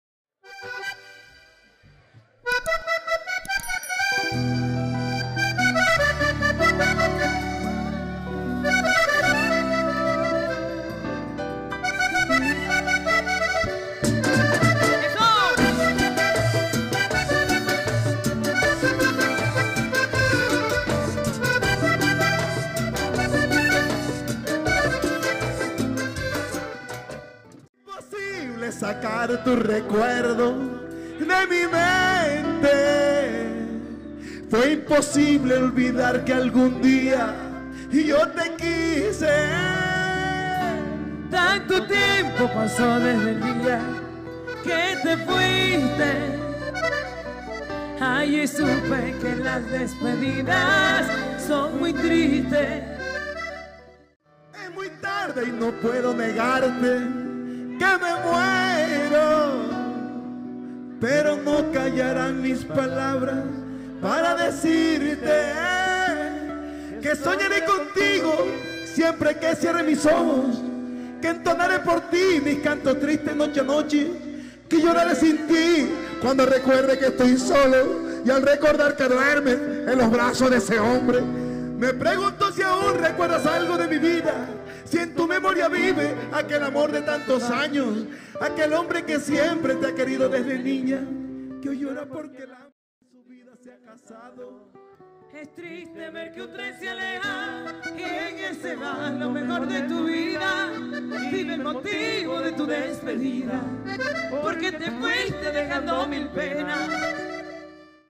LUTO EN LA MÚSICA VALLENATA MUERE EL ACORDEONERO Y COMPOSITOR Omar Geles Descanse en Paz
LUTO EN LA MÚSICA VALLENATA MUERE EL ACORDEONERO Y COMPOSITOR Omar Geles